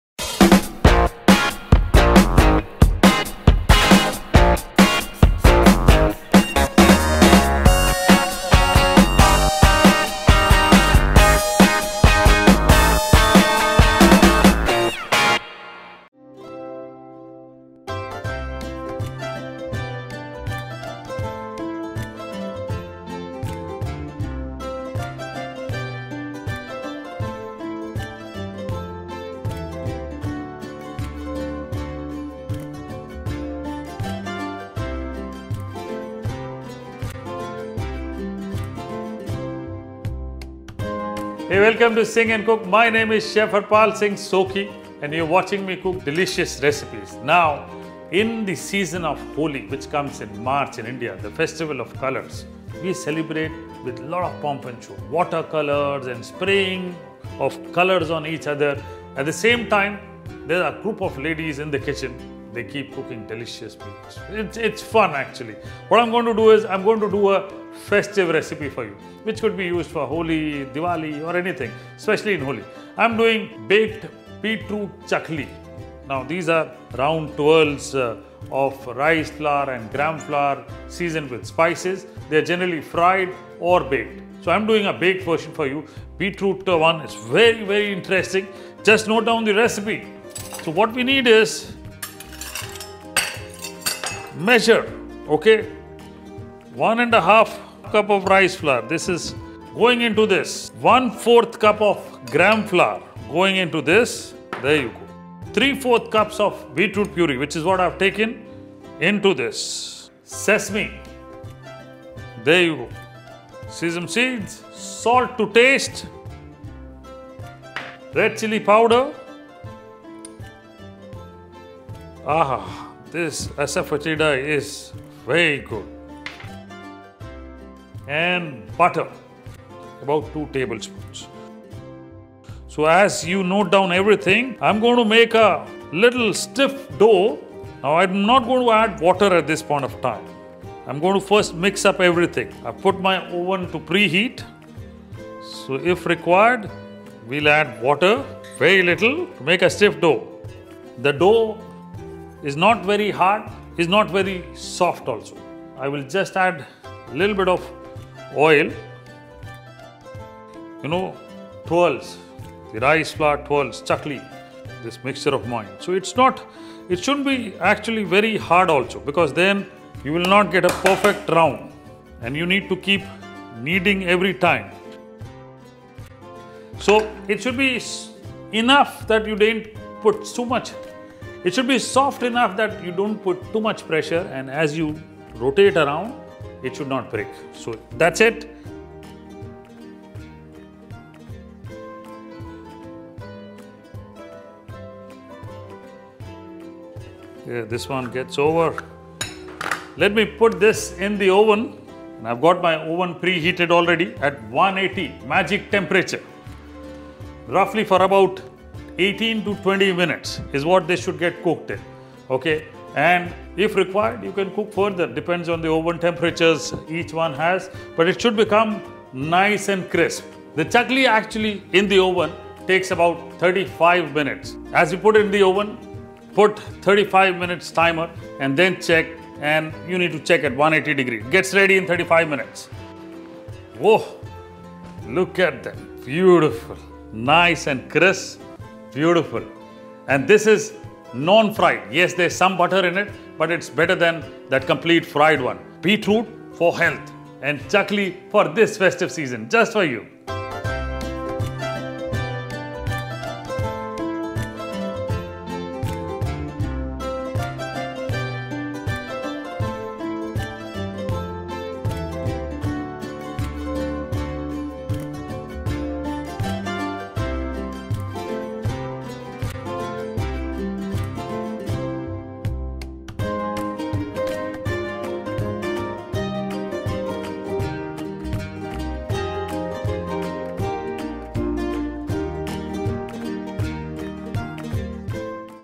Baked Beetroot chakali- Snacks
A South Indian Snack which is generally fried and served with tea. This one is a healthier option with the goodness of beetroot and is not fried, go ahead and enjoy..

Ingredient     Quantity
Beetroot Puree     ¾ cups
Sesame seeds (white)   ¾ tsp
Butter (room temperature)  2 Tbsp
Salt       1 tsp
Oil                for basting

Method:
1. In a mixing bowl add rice flour, gram flour, beetroot puree, sesame seeds, salt, red chili powder, Asafoetida, butter, mix well and make stiff dough. Add water and oil as required.
2. Pre heat the oven at 180* C
3. Place star shape disc in the chakali maker, Fill the chakali maker with some dough with star hole plate.
4.  In a baking tray place silicon mat and shape the chakali
5. Bake the chakali for 18-20 mins
6. Once done let the chakali cool, store in air tight container.